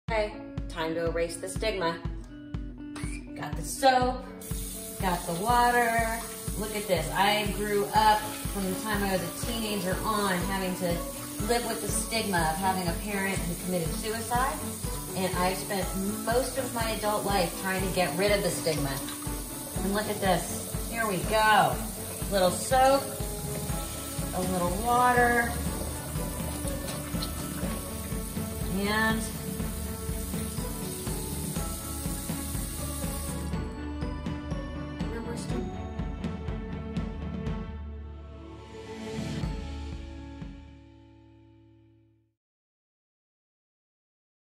ErasingTheStigma - Melissa Rivers
As a part of our Erasing The Stigma campaign, we’ve launched a social media challenge- ErasingTheStigma / StoppingTheSpread. Visit ErasingTheStigma.org to join us!